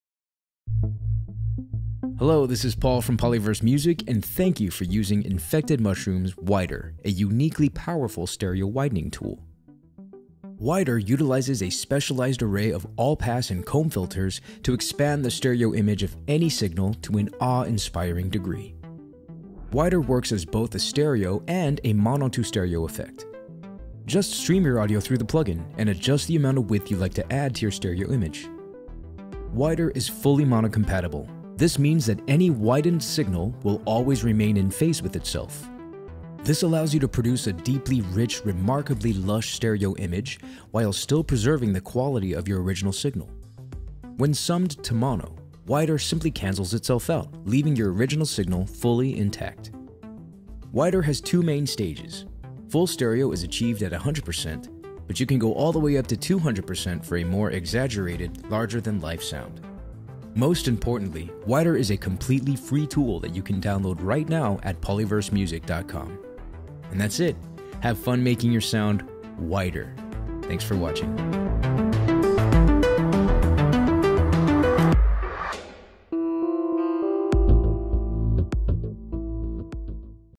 WIDER: How It Works
A short walkthrough on how to use Wider, and the amazing technology that keeps signal sent through Wider in phase every time.